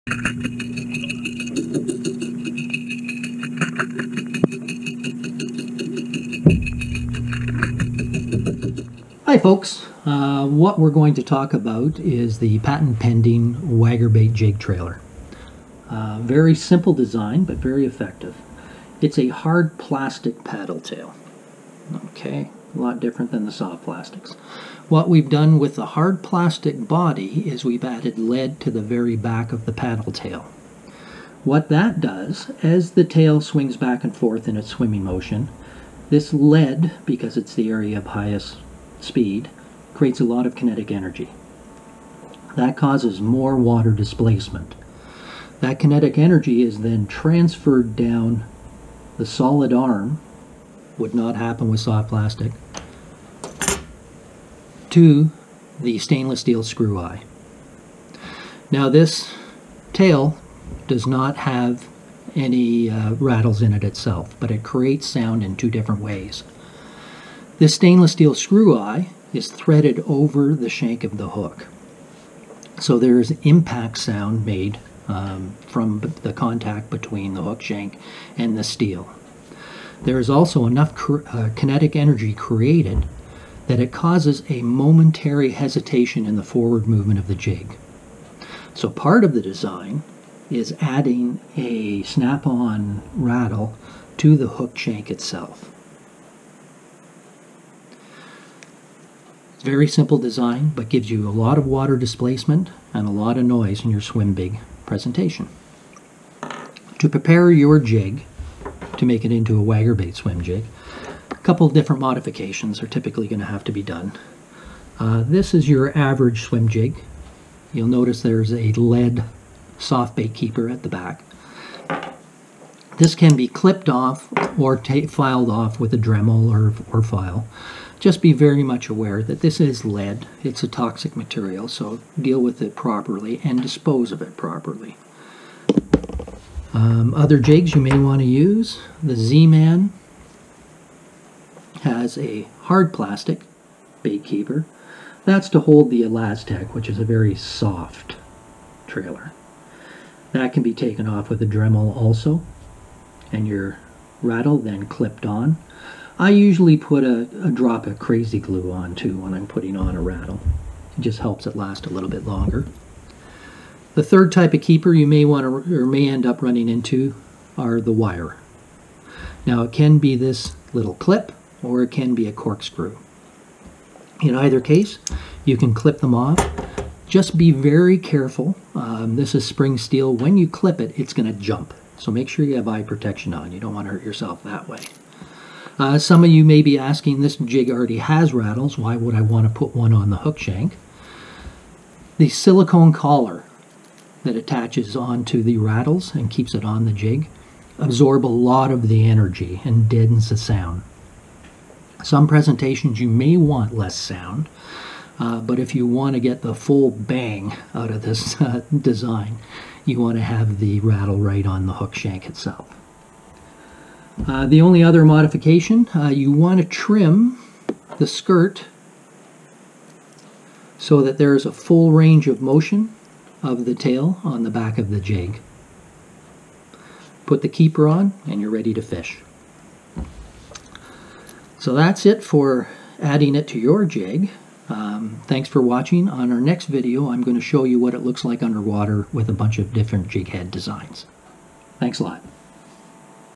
Waggerbait™ Jig Trailer - how it works and sets up.
Now you can turn your favorite bass jig into a Waggerbait™ swim jig. Get greater water displacement and more noise out of the swim jig presentation.